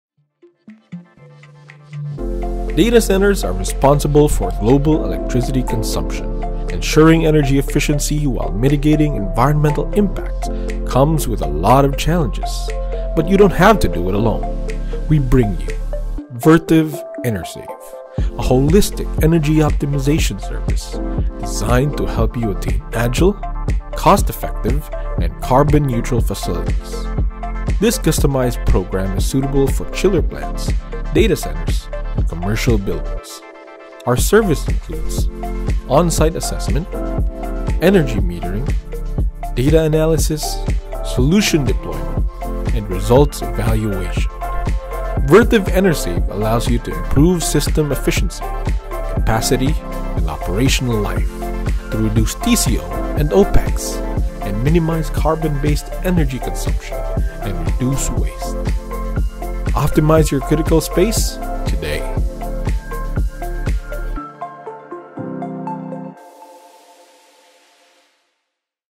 Thermal Infrastructure Optimization and Energy Saving Solution | Vertiv™ EnerSav
Thermal Infrastructure Optimization and Energy Saving Solution, The Vertiv™ EnerSav is an energy-saving optimization service program for any type of chiller and thermal infrastructure in data centers, chiller plants, and commercial buildings. This practical cost-effective solution, powered by IoT/AI technology and Vertiv’s thermal management products and monitoring solutions, aims to help facility managers conveniently manage and maximize the performance of critical thermal equipment while saving on energy, reducing their operational costs, and mitigating their environmental impacts.